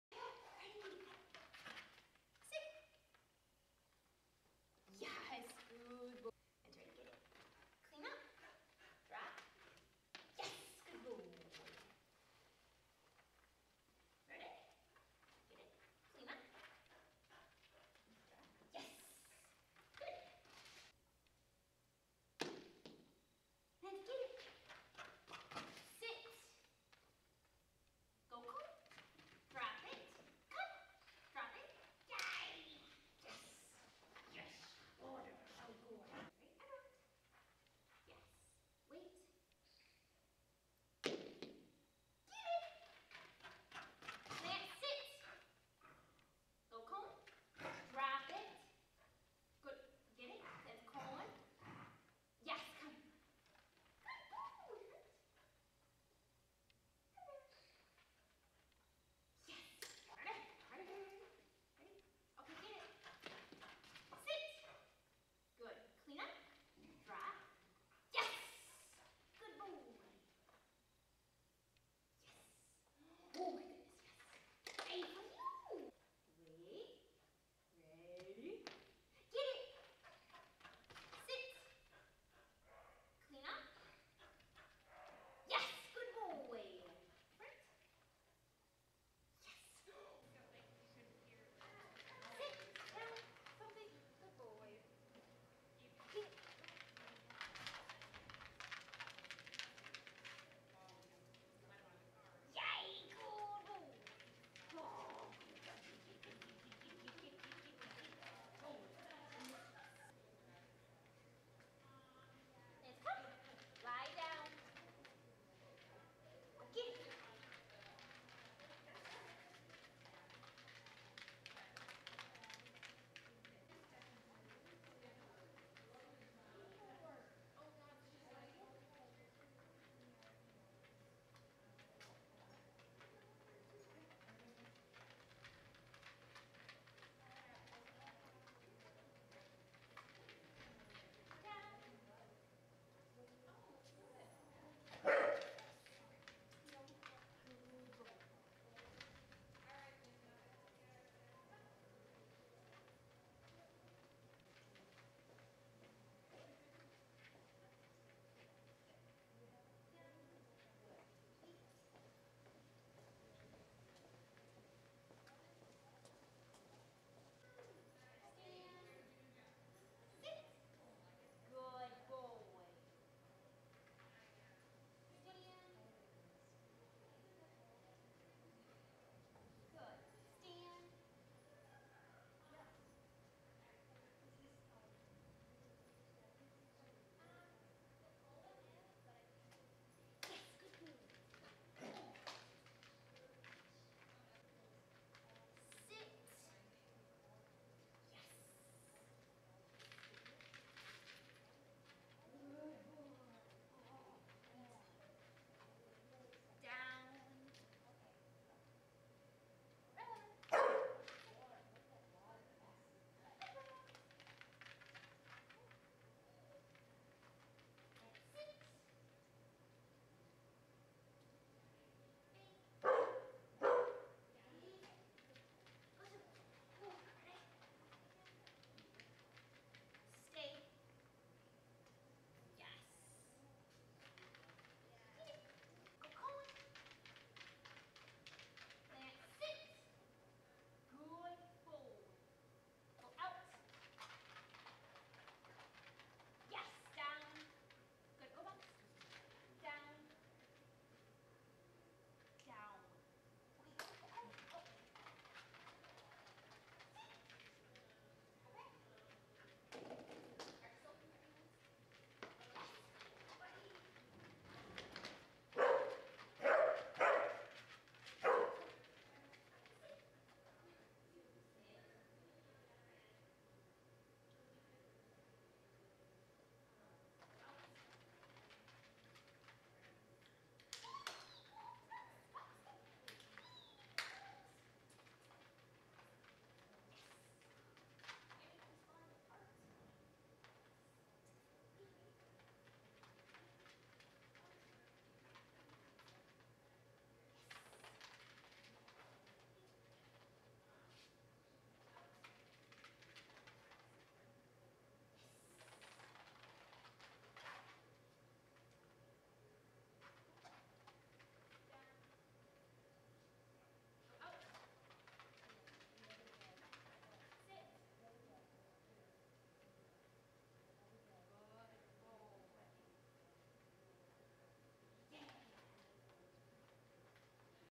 10-16-13 Lance the Corgi Advanced Teamwork practice
Practicing exercises for AKC's Advanced Teamwork class.